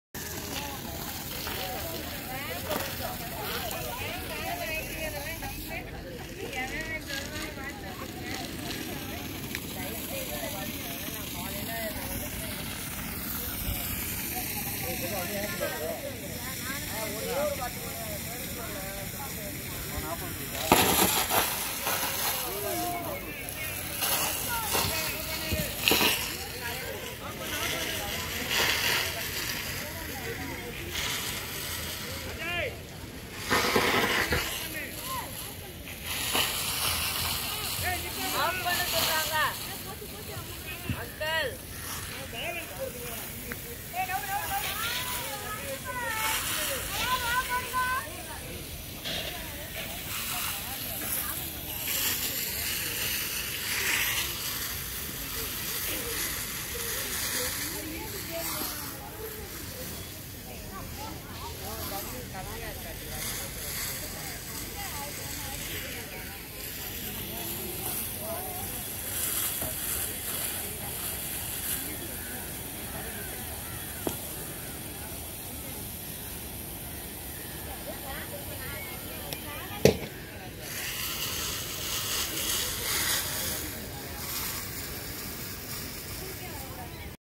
Anna Stadium Hockey Astroturf Well maintained by Hockey players and Association With own Expense
Hope Government  Helps in construction  of Well groomed Stadium around with Toulet Facilities.Approval still on ,request from Hockey Lover on 29th August Major Dyanchand  Birth Anniversary National  Sports Celebration  Day.Lets together uplift our Game.The Game on which mostly All age group is involved ,Poor to Rich,No caste ,No creed, players Patriotic by Nature.Women are Still Achieving spending their own Money to keep the Game Alive at any age.